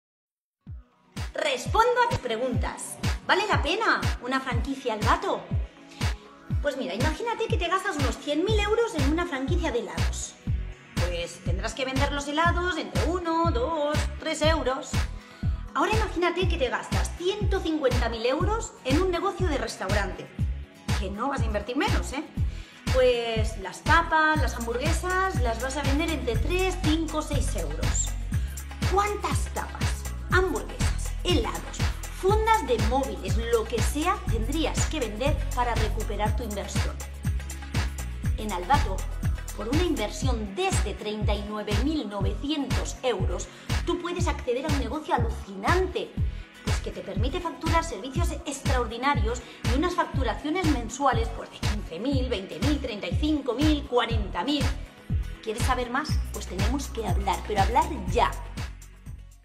RESPONDO A TUS PREGUNTAS. ¿ VALE LA PENA UNA FRANQUICIA ALVATO LUXURY DETAILNG?
En este video, Alicia Coronel, responsable del Departamento de Expansión Alvato, nos comenta si VALE LA PENA Invertir y trabajar en una franquicia de Alvato Luxury Detailing.

Este video es buenísimo.